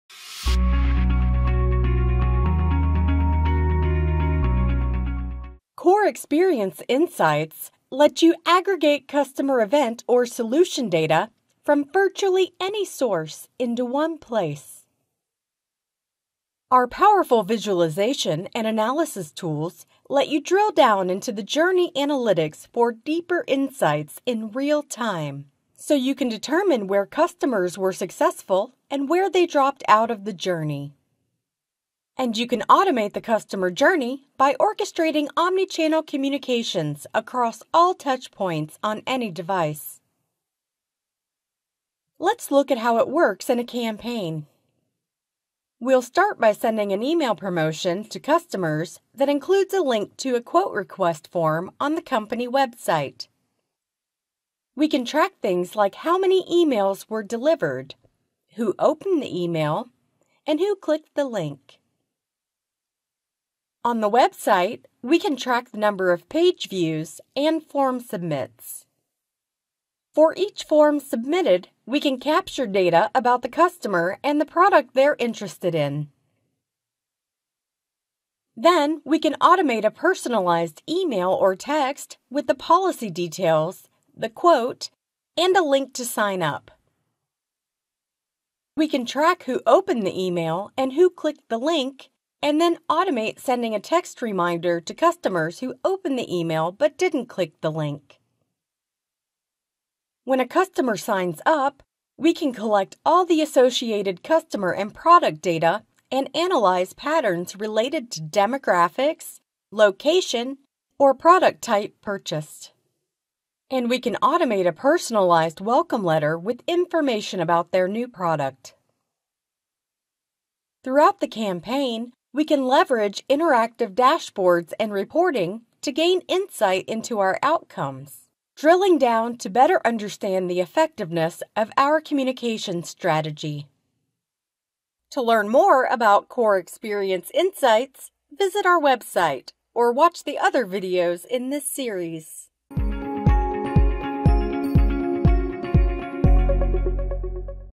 OpenText Core Experience Insights
Core Experience Insights lets you aggregate customer event or solution data from virtually any source into one place. Our powerful visualization and analysis tools let you drill down into the journey analytics for deeper insights in real time, so you can determine where customers were successful and where they dropped out of the journey. And you can automate the customer journey by orchestrating omni-channel communications across all touchpoints on any device.

Watch this video to learn more about the benefits of Core Experience Insights and how it can work for your company to improve your customers’ experience with your brand.
0:05 – Benefits of Core Experience Insights
0:42 – Example of how Core Experience Insights works in a campaign

If you find this video useful, please like or comment.

This video was created using the default UI of Core Experience Insights 21.3. What you see in this video might not reflect what you see in your version or customized interface.